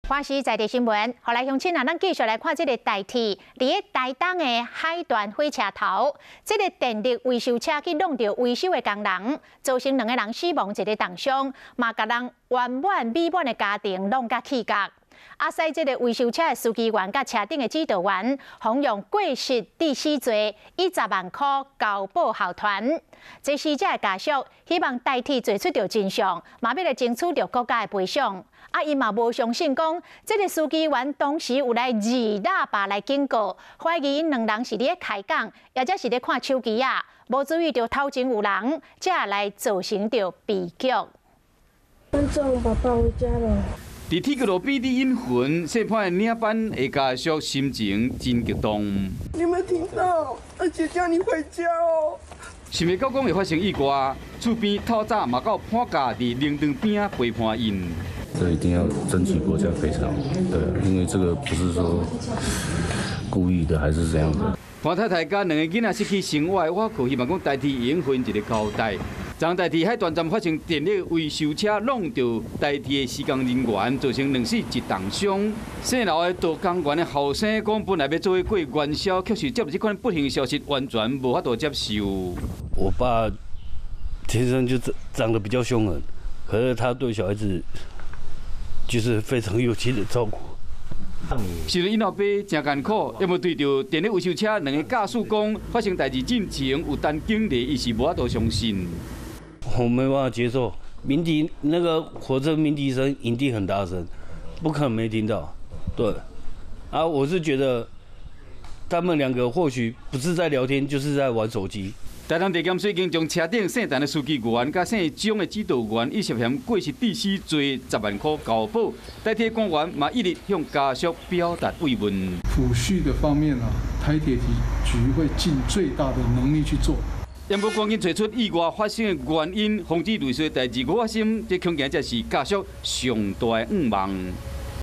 【台語新聞】台鐵道工班2死1傷 2司機員10萬交保｜華視台語新聞 2021.02.24
台鐵海端車站電力維修車撞上自家維修工人，造成兩死一重傷，車上的陳姓司機員及鍾姓指導員，被依過失致死等罪，以10萬元交保候傳。身亡的潘姓領班家屬希望台鐵能找出真相，並且要爭取國賠。劉姓道工員的兒子不相信司機員曾鳴笛示警的說法，也懷疑他們可能是在聊天或看手機，沒注意到前方有人，才釀成悲劇。